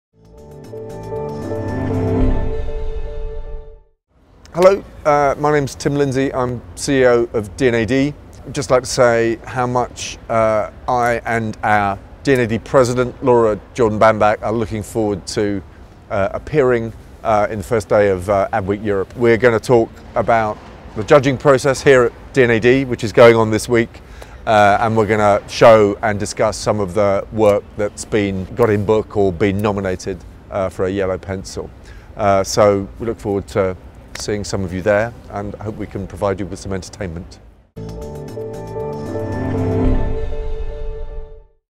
Join D&AD at Advertising Week Europe
D&AD will be holding 2 talks at Advertising Week Europe. We'd love you to join us, here's the details